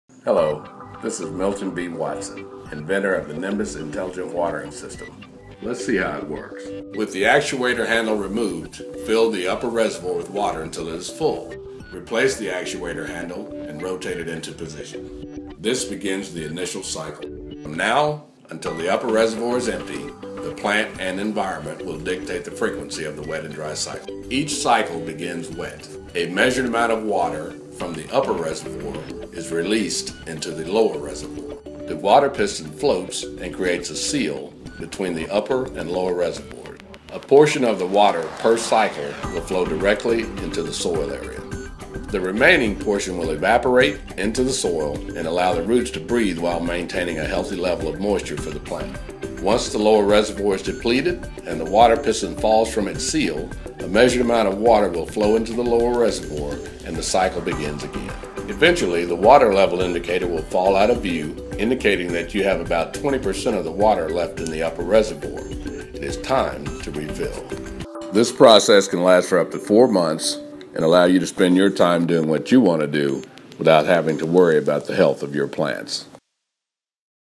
NIMBUS: How it works.
Inventor Milton B. Watson takes you through how the NIMBUS works.